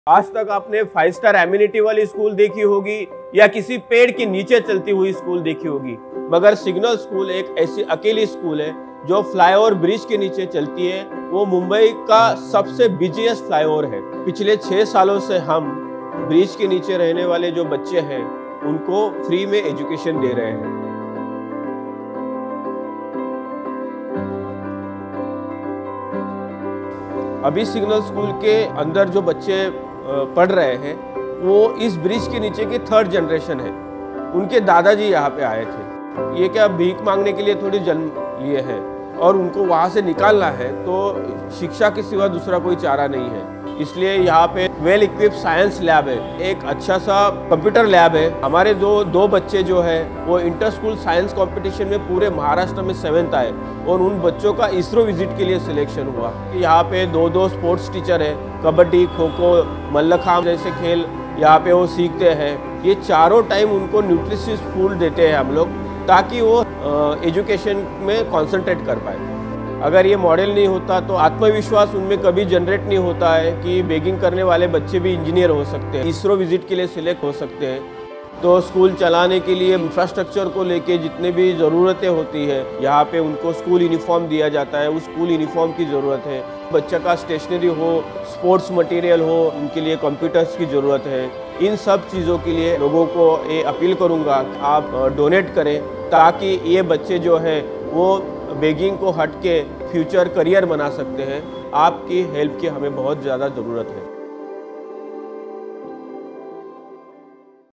Help Bhatu Sawant Transform The Lives Of Hundreds Of Street Children
Signal Shala in Thane is working relentlessly to give countless street children a life full of better opportunities and a bright future.